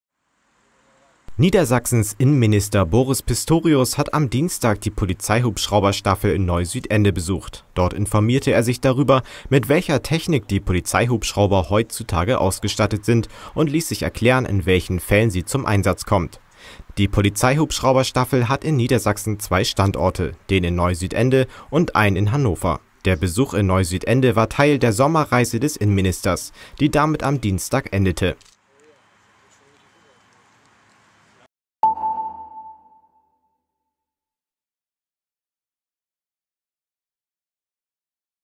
Boris Pistorius besucht Polizeihubschrauberstaffel
Neusüdende, 01. August 2017: Niedersachsens Innenminister Boris Pistorius hat am Dienstag die Polizeihubschrauberstaffel in Neusüdende besucht.